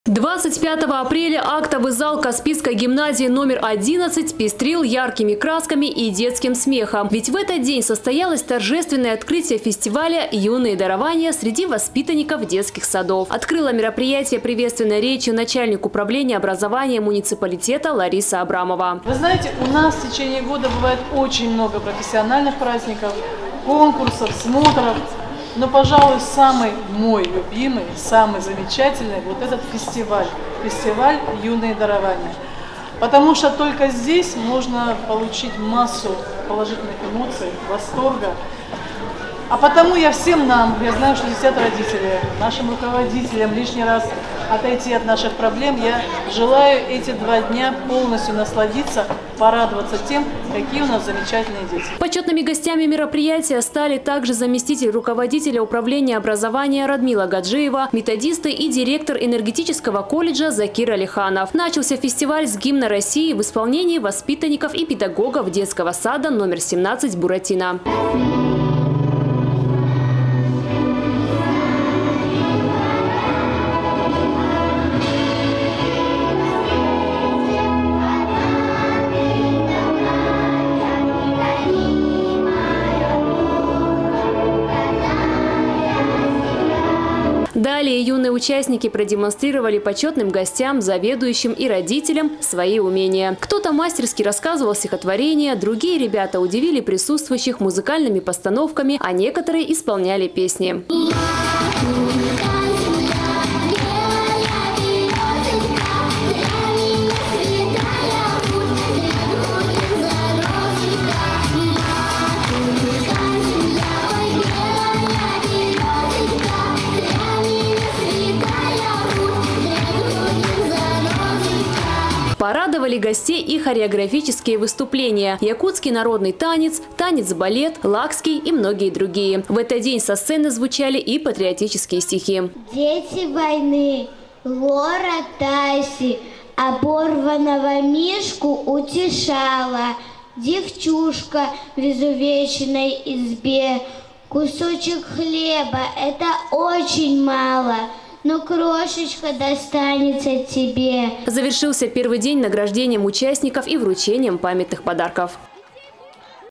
Фестиваль «Юные дарования» стартовал в Каспийске
25 апреля актовый зал Каспийской гимназии номер 11 пестрил яркими красками и детским смехом, ведь в этот день состоялось торжественное открытие фестиваля «Юные дарования» среди воспитанников детских садов. Открыла мероприятие приветственной речью начальник управления образования муниципалитета Лариса Абрамова.

Почетными гостями мероприятия стали также заместитель руководителя управления образования Радмила Гаджиева, методисты и директор Энергетического колледжа Закир Алиханов.